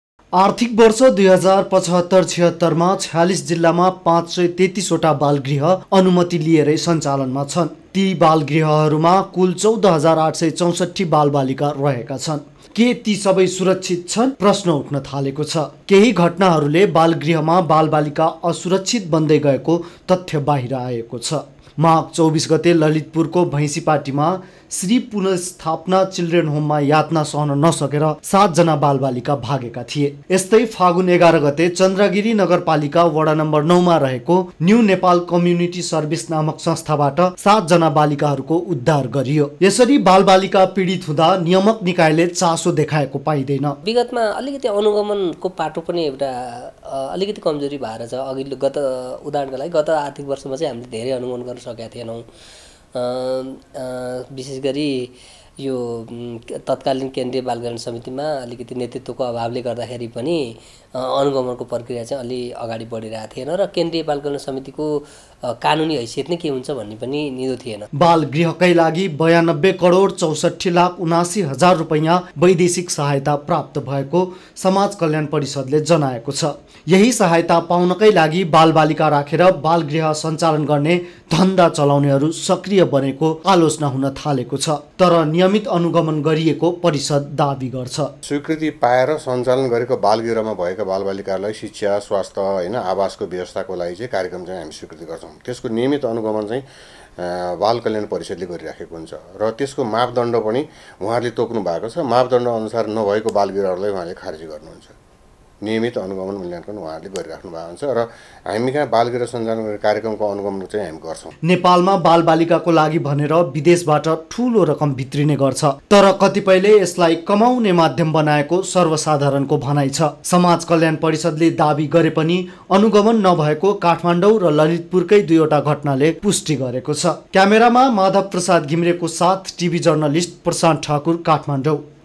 के बालगृहमा रहेका बालबालिका सुरक्षित छन् ? - NEWS24 TV
बालगृह असुरक्षित बन्दै गएको एक तथ्यांकले देखाएको छ । काठमाडौं उपत्यकामा पछिल्लो पटक भएका दुई घटनाले पनि बालगृह असुरक्षित रहेको पुष्टि गर्छ । सरकारले पनि बालगृहको नियमित अनुगमन गर्न सकेको छैन । बालगृहलाई कतिपयले कमाउने माध्याम बनाएका छन् ।